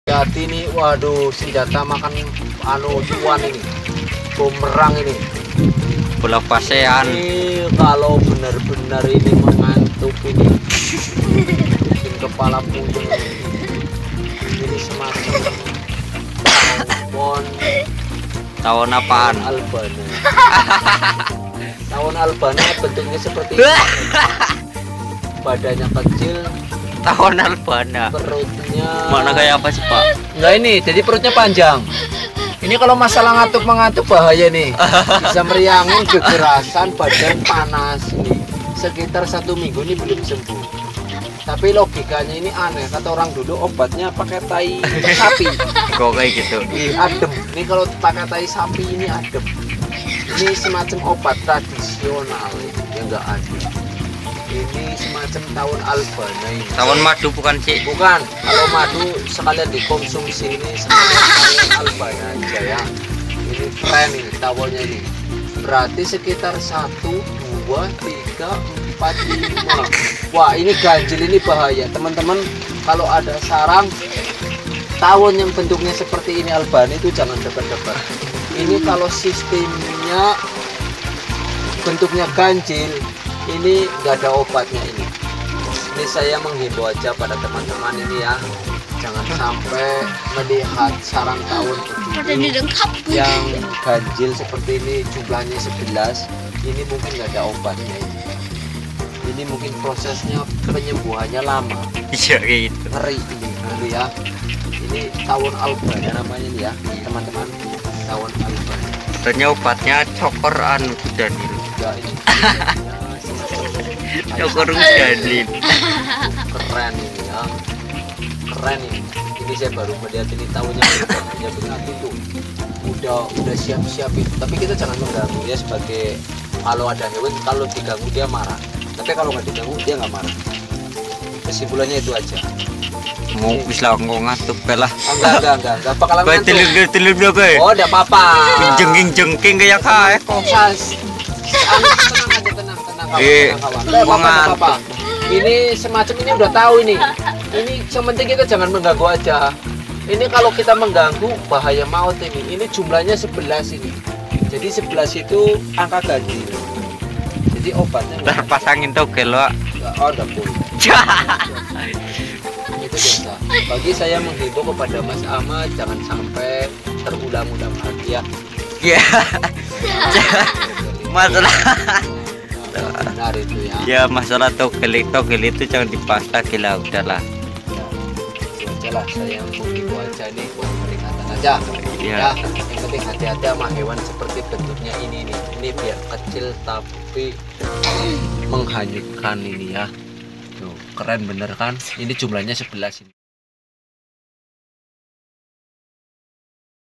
Tawon Al Bana//jika anada tersengat oleh jumlah tawon yang ganjil hati hati..!!!!
di vidio ini hanya utuk hiburan semata yang sebenarnya itu bukan tawon al-banna melainkan adalah
              Tawon Kertas

Bertubuh ramping dan perut panjang, tawon kertas (paper wasps) lebih berbahaya dibandingkan yellow jacket atau tawon jaket kuning.Mereka membentuk sarang seperti terbuat dari kertas. Biasanya di bawah atap atau langit-langit bangunan.
Asal tak diagnggu, mereka tidak akan menyerang meski sarang tersebut berdekatan dengan manusia. Tawon ini mampu memangsa larva atau hama di kebun

jadi di tentang masalah ganjil atau genap nya jumlah tawon tersebut seperti yang ada di video itu adalah non fakta atau hoax jadi tawon dikatakan berbahaya ya itu karena jenisnya bukan karena ganjil atau genap nya karena video ini hanya untuk hiburan Semata Jadi mohon maaf atas segala kekurangan